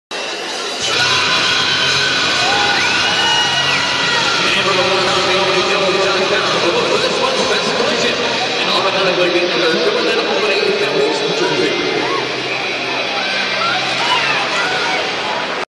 Albany Devils, H&V Collision of the Game (Oct. 21)
Each Devils home game will include a "H&V Collision of the Game" at the end of each month fans can go to thealbanydevils.com and vote on the "H&V Collision of the Month" By voting you automatically have the chance to win and Albany Devils jersey.